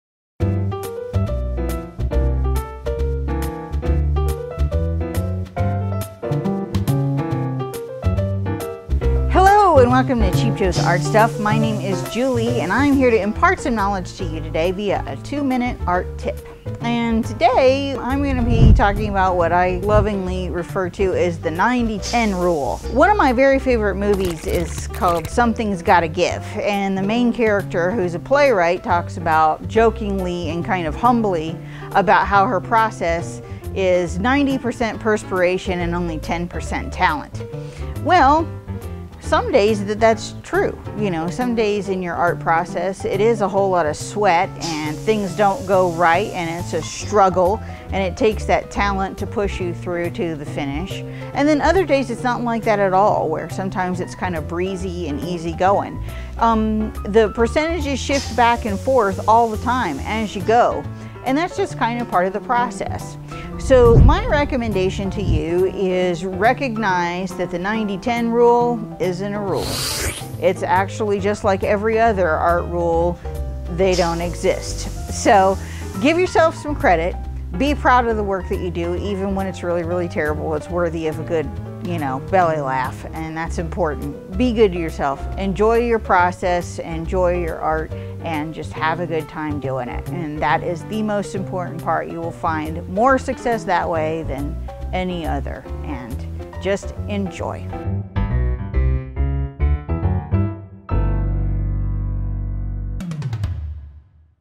Cheap Joe's 2 Minute Art Tip - The 90/10 Rule
It's another free Cheap Joe's 2 Minute Art Tip with 2 Minute Julie. In this one, Julie talks about the 90/10 rule.

Make more art and spend less money by shopping at Cheap Joe's Art Stuff.